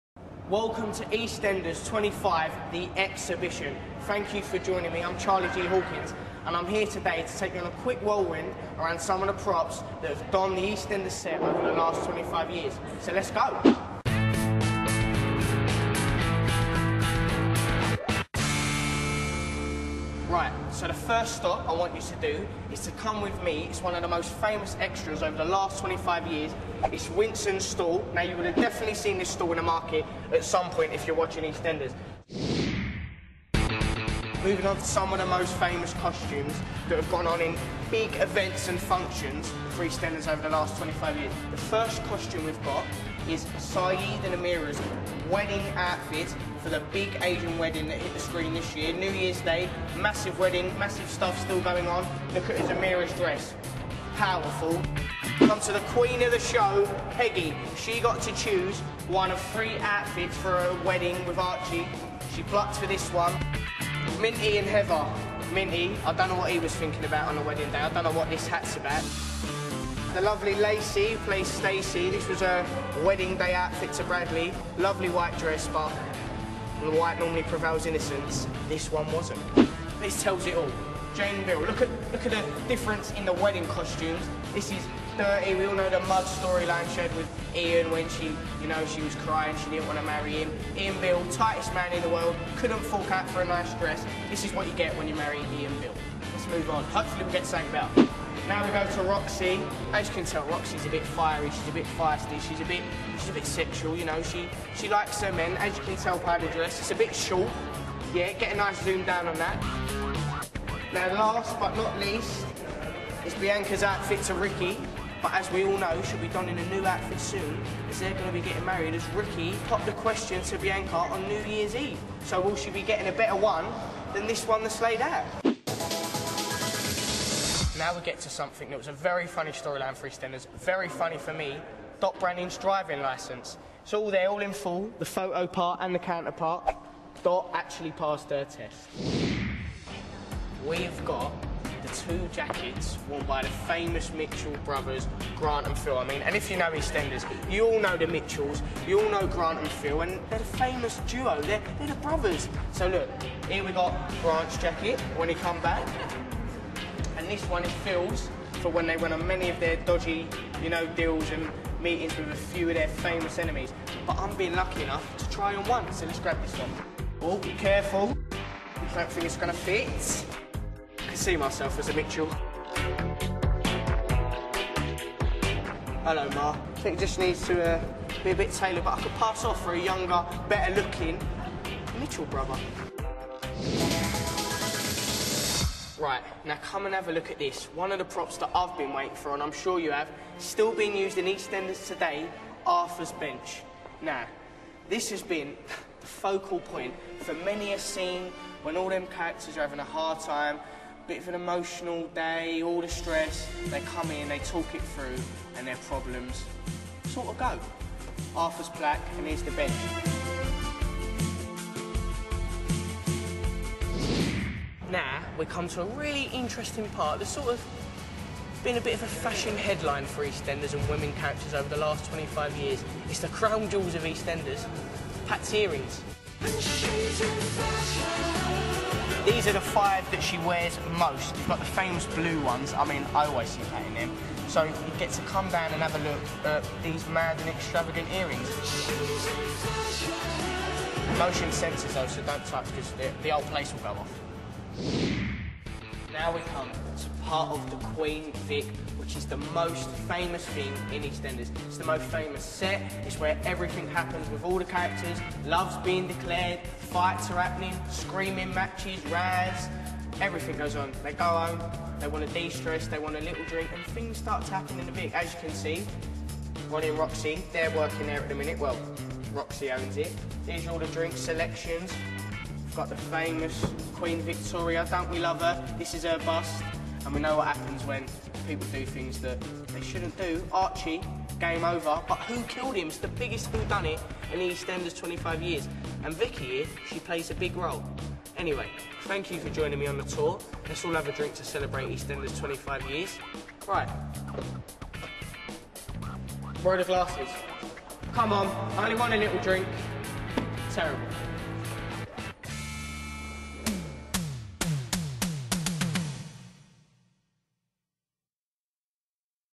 EE25: The Exhibition tour video
Happy cockadoodle birthday EastEnders! Darren Miller could almost be a Beale in the making, what with his money-making ideas and cheap suit. Thank the lordy that actor, Charlie G Hawkins is nothing of the sort. Big C was on hand to take you on a jubbly tour of our 25th anniversary exhibition. So, if you missed out on tickets - worry not... this is even better. Rest your legs with a cuppa and a hob nob...